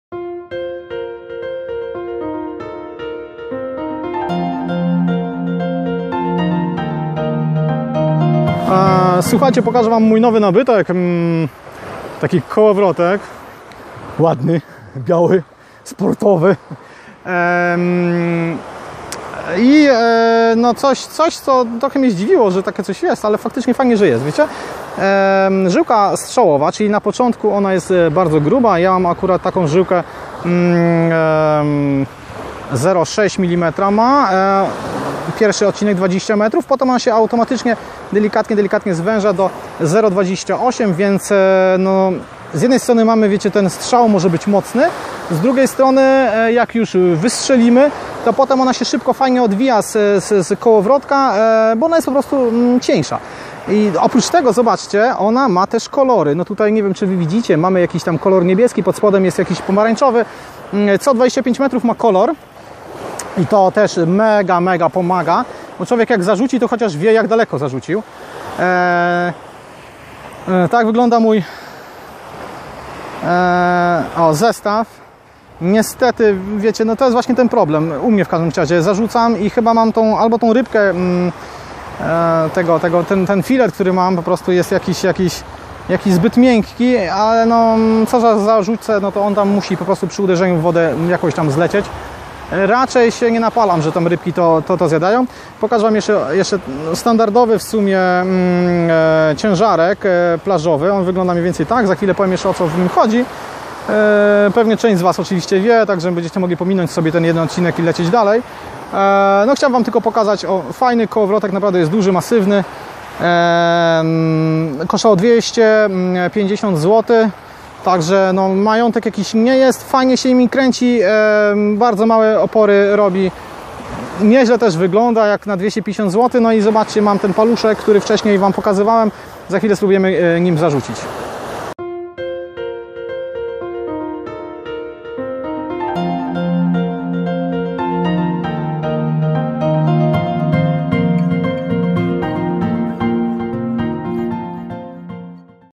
WladekTV - Kołowrotek i kolorowa żyłka strzałowa
Dość dobry kołowrotek i wędzisko, ale wędkarz kiepski. Muszę potrenować zarzucanie i powinno być spoko :) Na razie zapraszam do zapoznania się z filmem, na którym przedstawiam pokrótce kołowrotek oraz nieco więcej czasu poświęcam żyłce...